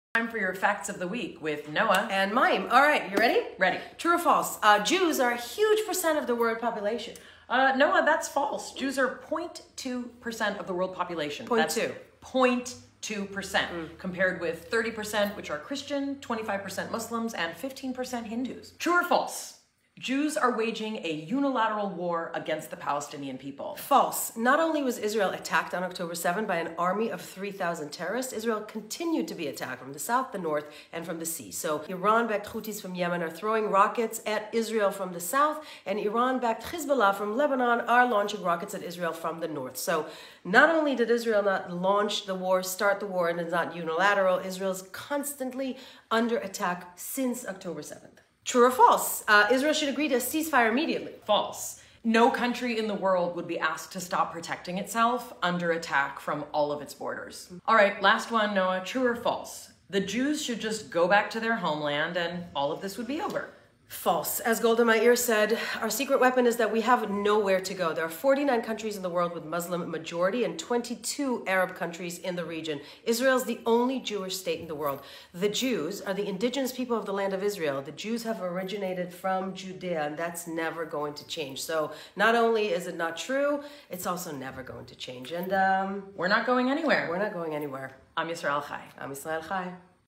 Facts of the Week with Mayim Bialik and Noa Tishby| Israel at War
TRUE OR FALSE With Mayim Bialik and Noa Tishby @MayimBialik

Jews are a huge percent of the world population?
FALSE. Jews are 0.2% of the world population, compared with 30% which are Christian, 25% Muslims, and 15% Hindus.

Jews are waging a unilateral war against the Palestinian people?
FALSE. Not only was Israel attacked on October 7th by an army of 3,000 terrorists, Israel continued to be attacked from the south, the north, and from the sea. So Iran-backed Houthis from Yemen are throwing rockets at Israel from the South, and Iran-backed Hezbollah from Lebanon are launching rockets at Israel from the north. So not only did Israel not launch the war, start the war, and it’s not unilateral. Israel’s constantly under attack since October 7th.

Israel should agree to a ceasefire immediately?
FALSE. No country in the world would be asked to stop protecting itself under attack from all of its borders. The Jews should just go back to their homeland and all of this would be over? FALSE. As Golda Meir said, our secret weapon is that we have nowhere to go. There are 49 countries in the world with Muslim majority and 22 Arab countries in the region. Israel is the only Jewish state in the world. The Jews are the indigenous people of the land of Israel. The Jews have originated from Judea, and that’s never going to change.